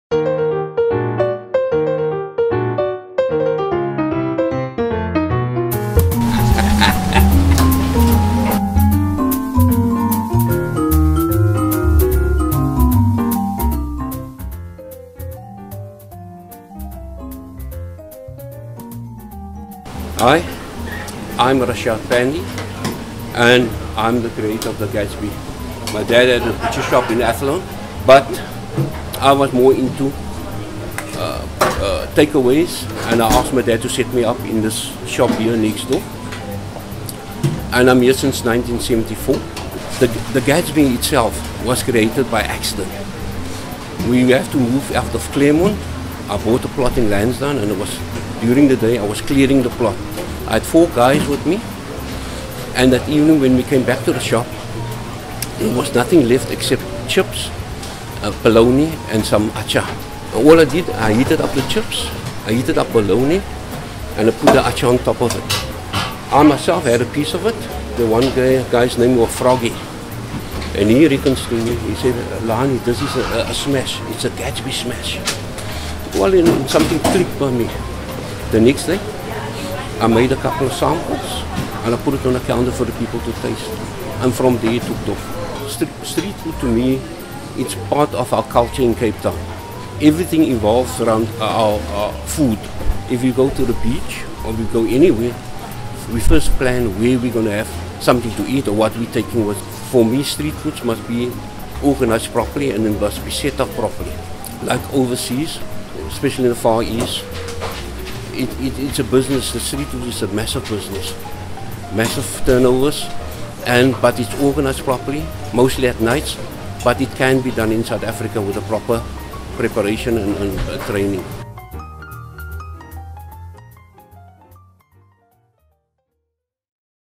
Meet the creator of South Africa's most iconic street food, the Gatsby
Eat Out heads to Super Fisheries in Athlone to meet Rashaad Pandy, the creator of South Africa’s iconic super sandwich, the Gatsby, and find out how the glorious recipe came about.